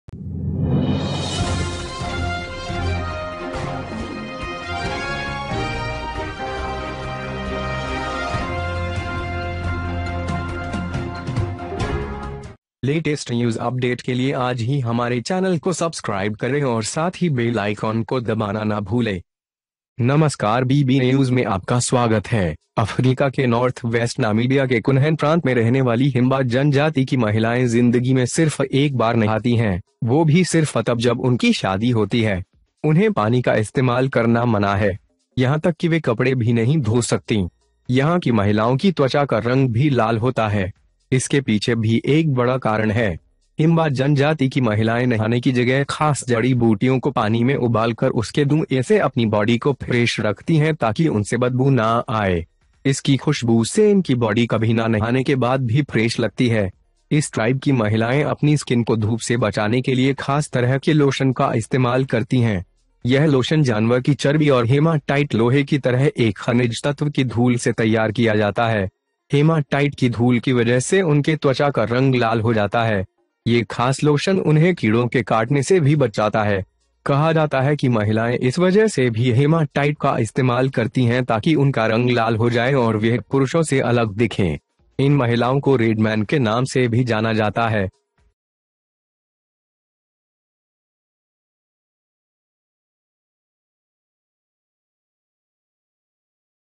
पूरी जिंदगी में सिर्फ एक बार नहाती हैं यहां की महिलाएं, ऐसे रखती हैं खुद को फ्रेश
The women of the Hemba tribe living in Kunan province of North-West Namibia, Africa, bathes only once in a lifetime, even when they get married. They are forbidden to use water. Even they can not wash clothes Here the skin of women's skin is also red. There is also a big reason behind this.

Women of the Huma tribe keep the body fresh in the bath instead of boiling them in water so that they do not smell. With its fragrance, its body seems fresh even after bathing.

Women of this Tribe use special lotions to protect their skin from sunlight. This lotion is prepared with the fat of the animal and the hematite (a mineral element like iron). Due to the hematite dust, the color of their skin becomes red. These special lotions also protect them from bites of insects.

It is said that women also use Hematite because of their color red and they look different from men. These women are also known as Red Man.

BB NEWS!!